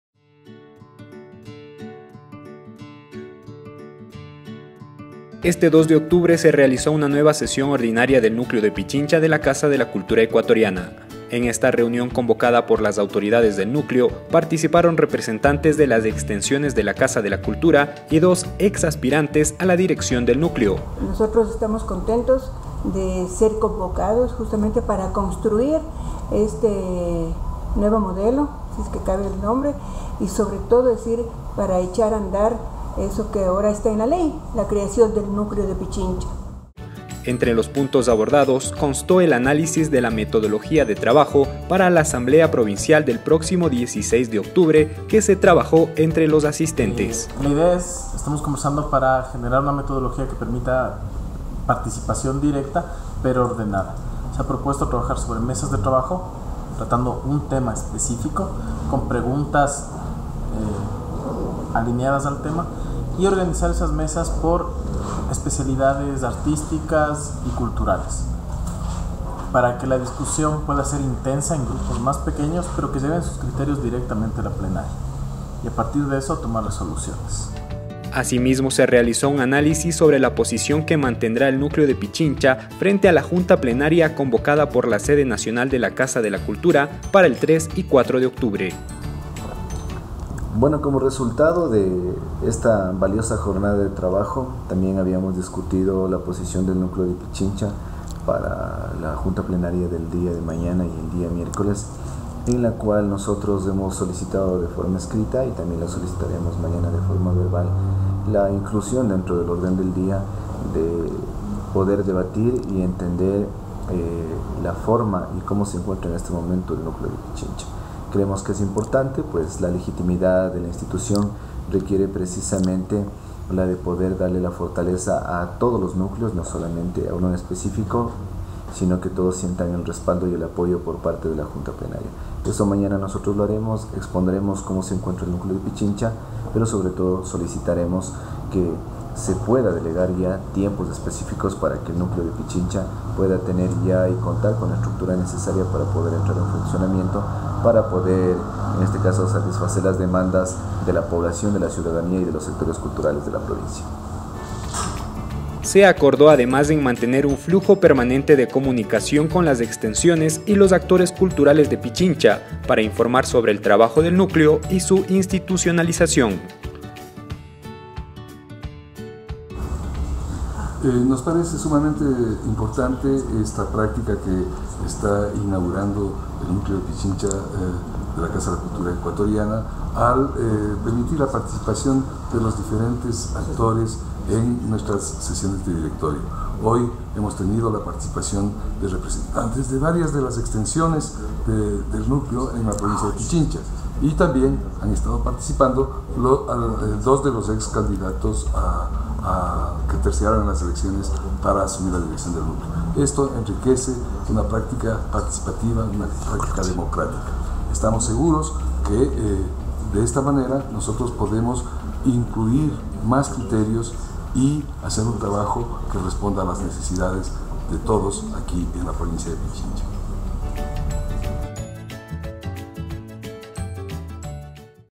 2017-10-02 Sesión ordinaria Directorio del Núcleo de Pichincha de la Casa de la Cultura Ecuatoriana.
Les contamos los resultados de la sesión ordinaria que mantuvo hoy el directorio del Núcleo de Pichincha de la Casa de la Cultura Ecuatoriana. Un enriquecedor diálogo que amplia la perspectiva de trabajo dentro de la provincia y frente a las instituciones culturales del país.